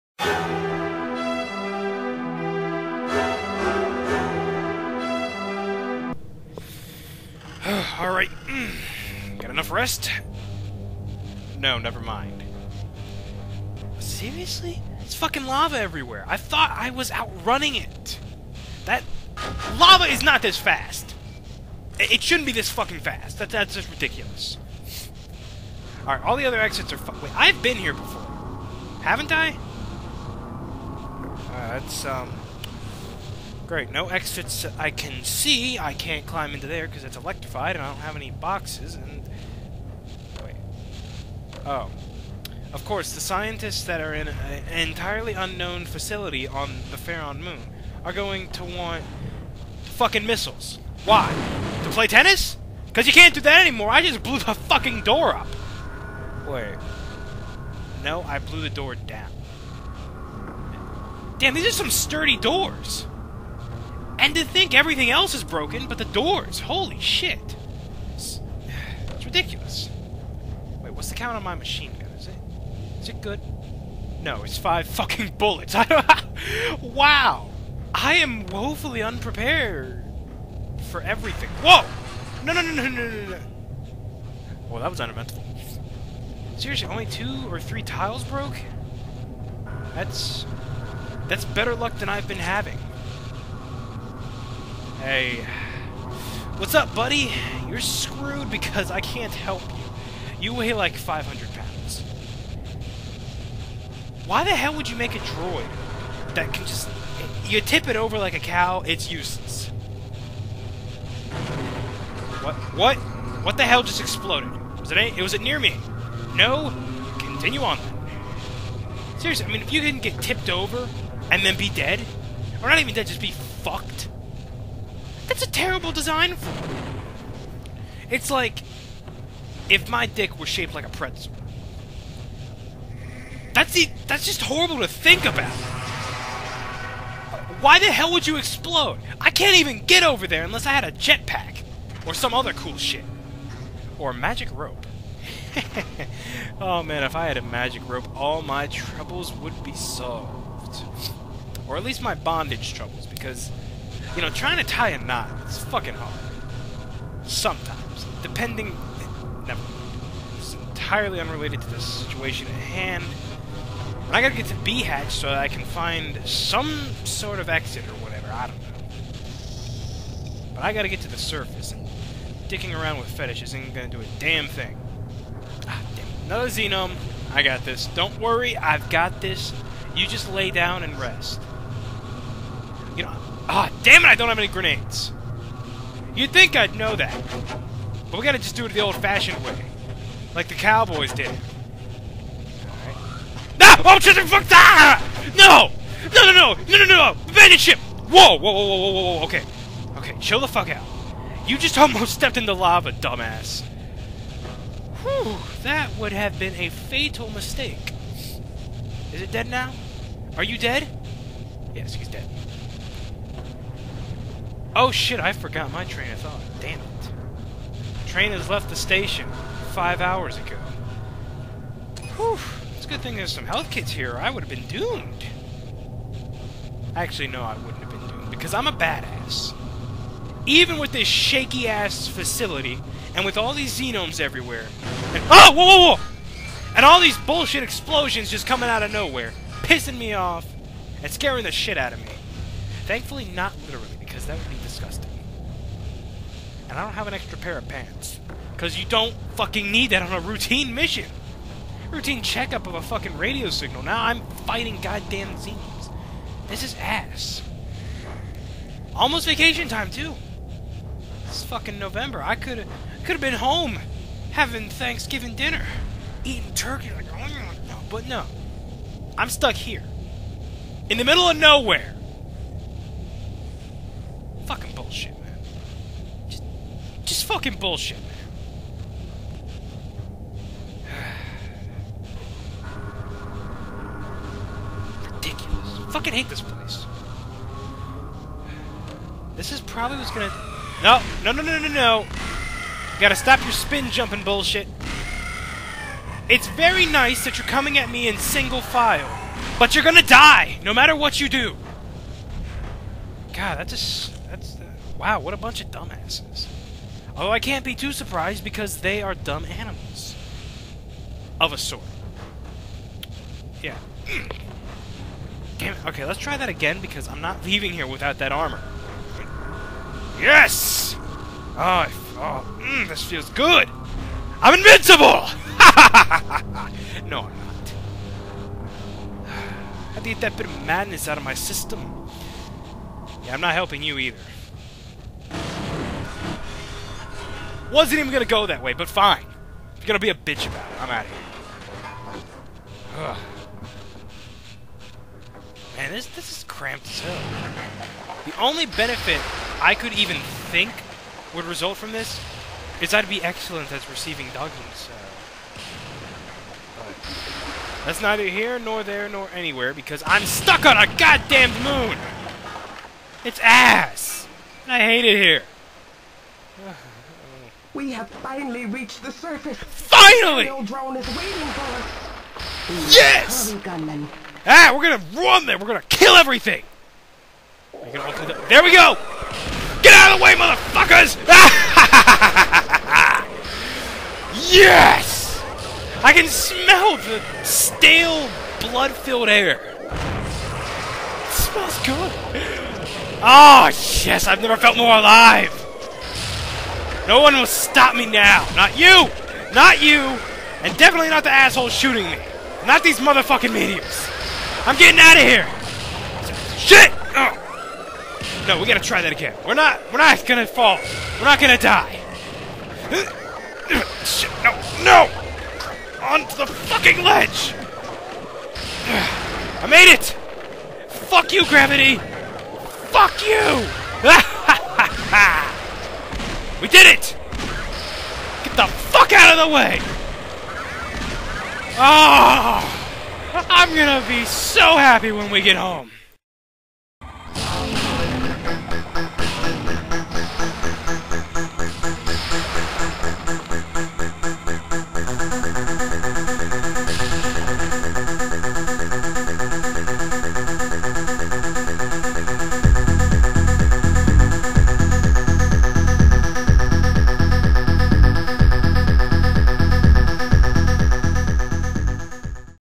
Archer's Mind Episode 28
The inner thoughts of Major Archer, fugitive from death and recent escapee. In this episode, Archer claws his way through the labs, unsuccessfully destroys a door, and reaches the aerial drone.